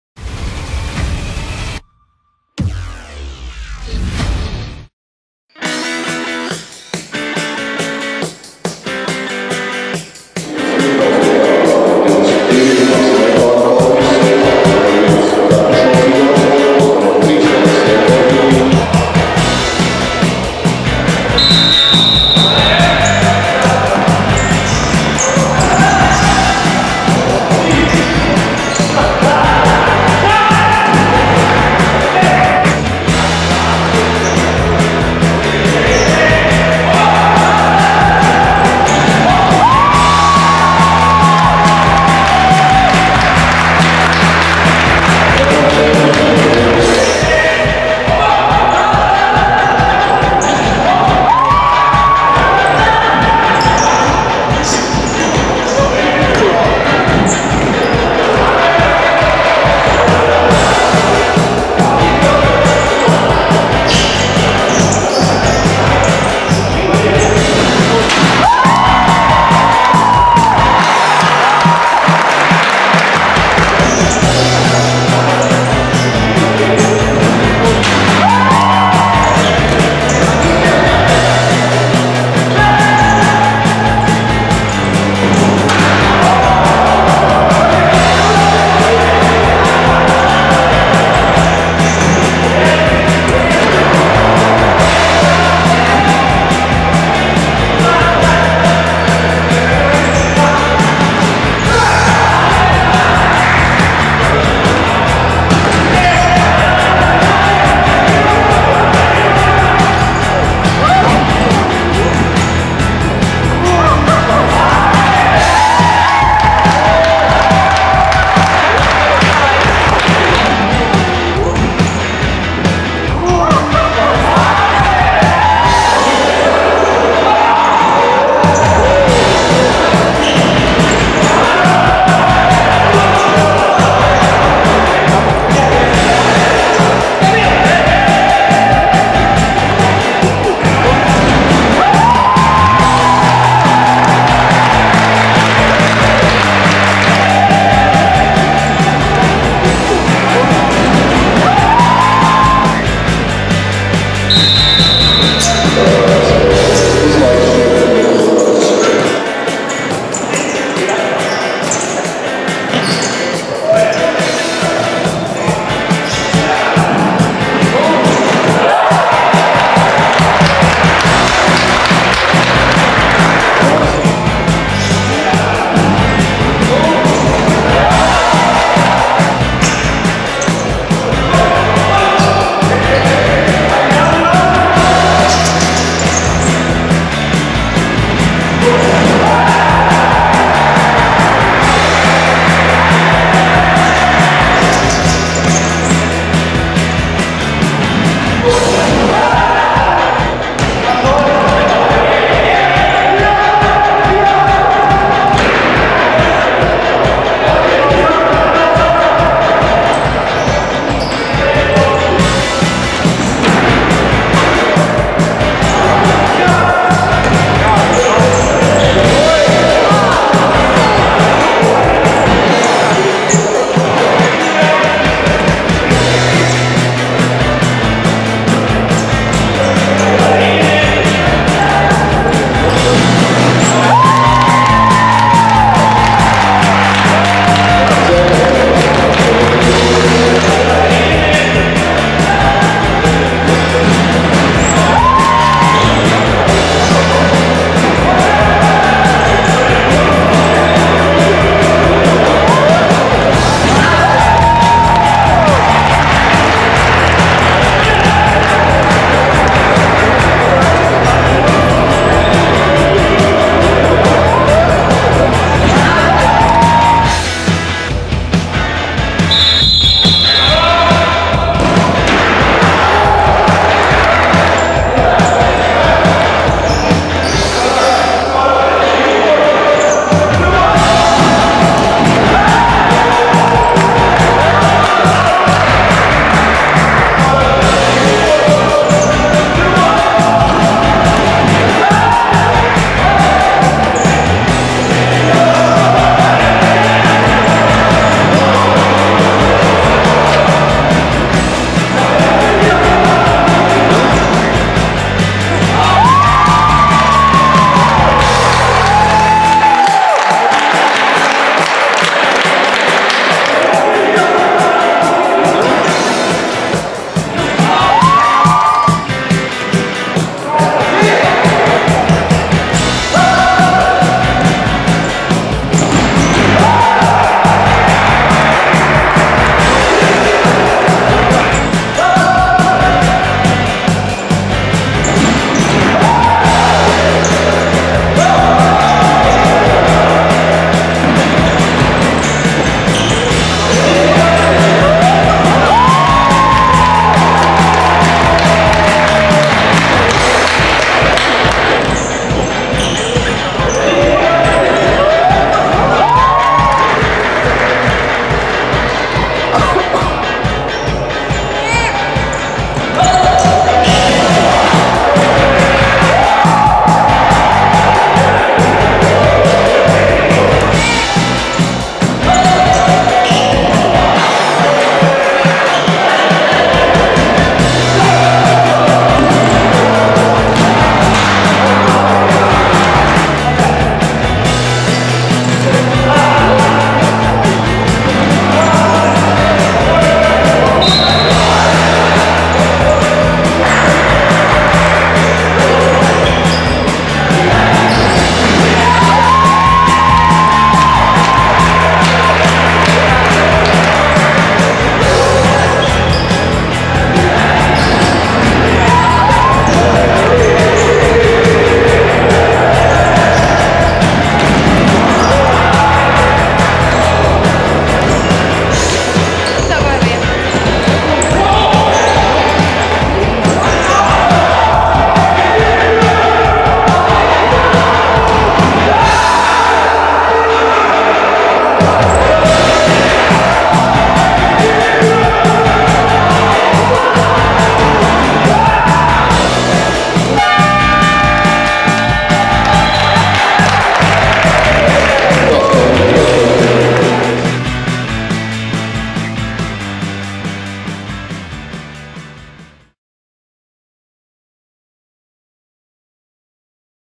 Grobiņas SC - Jako Sport/Graanul Pellets [LTFA 1. Līga 2019/20 Highlights]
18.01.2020. LTFA 1. Līga 9. kārta
Grobiņas SC - Jako Sport/Graanul Pellets 10:5